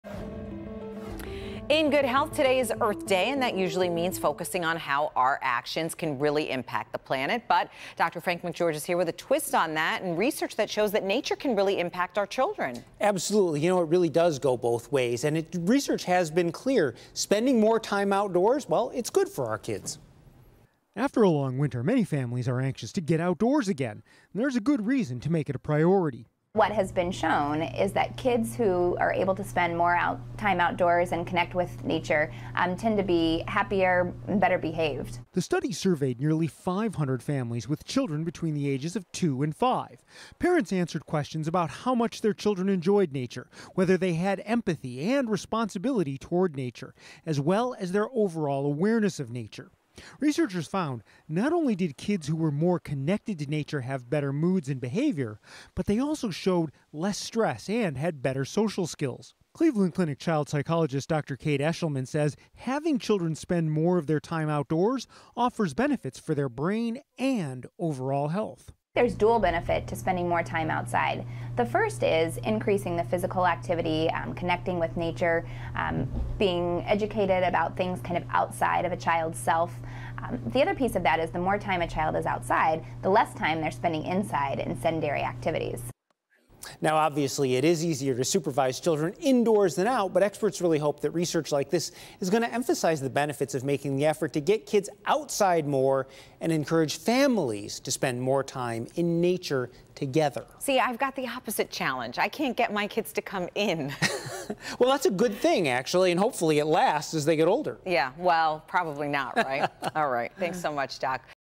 Good Health: Time in nature benefits children
Research is showing how nature can impact children.